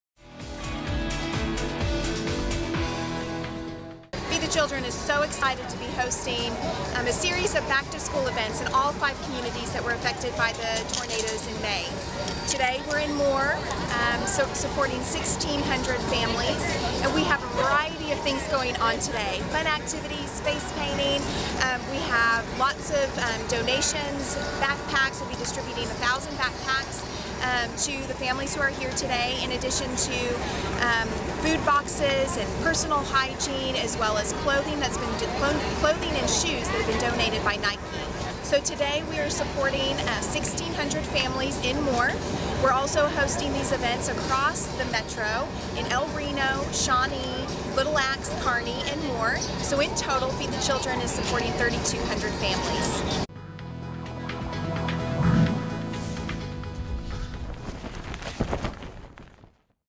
"Student Strong" in Moore (2013-08-15)
Feed The Children, along with community and business partners, host “Student Strong” back-to-school events in Carney, Moore, Little Axe, Shawnee and El Reno.